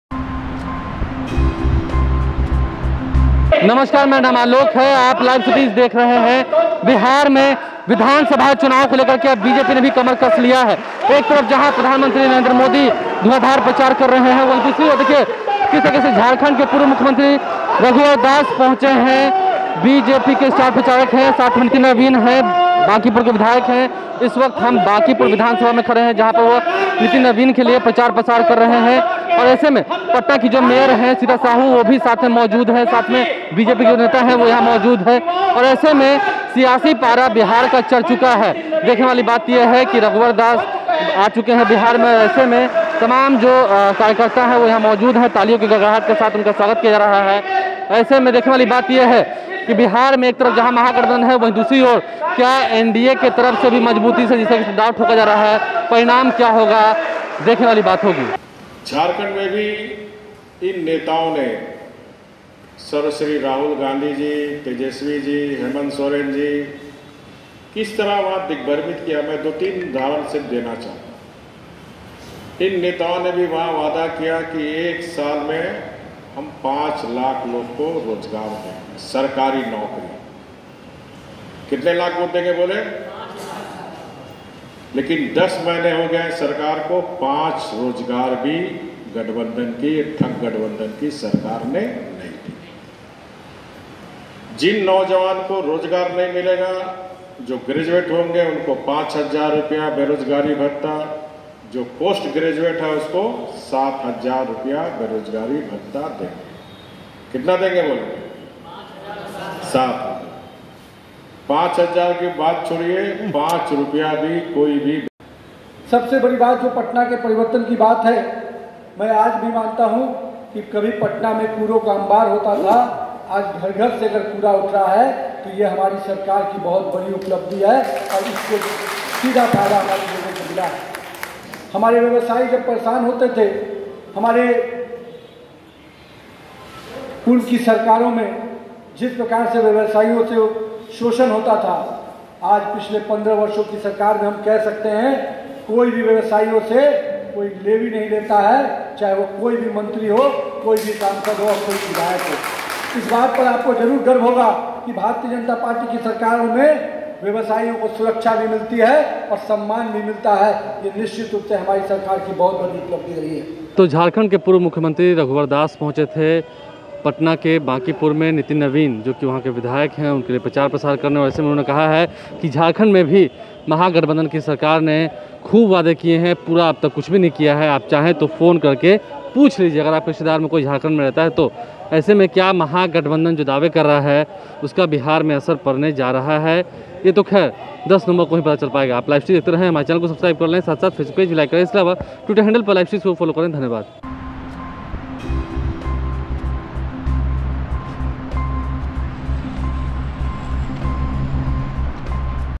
Bihar Chunav : पटना पहुंचे Raghubar Das ने लोगों को चेताया,बोले-वोट गलत जगह गया तो फिर आएगा गुंडाराज
Bihar Chunav : पटना पहुंचे Raghubar Das ने लोगों को चेताया,बोले-वोट गलत जगह गया तो फिर आएगा गुंडाराज | Bihar News | LiveCities